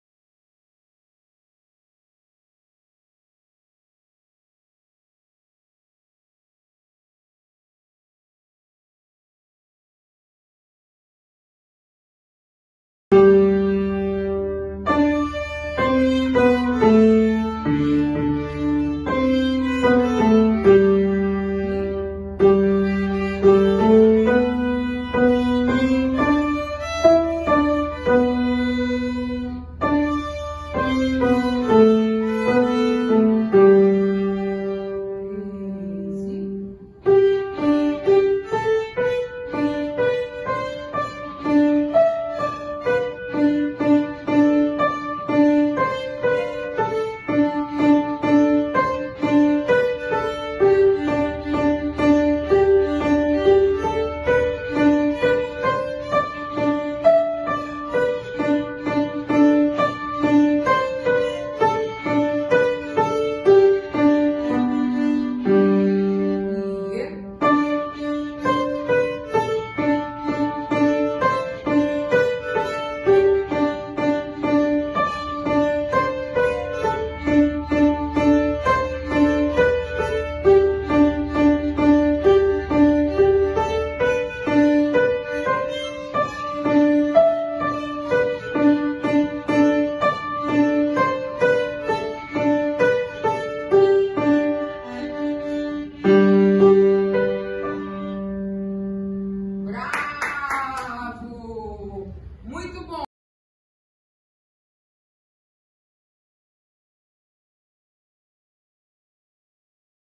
Feliz dia dos pais, com música! (V/ 380.2022)
motivacional
Local: Auditório Escola de Música
Professora violino : Raquel Ferreira
Aluna: amiga Glória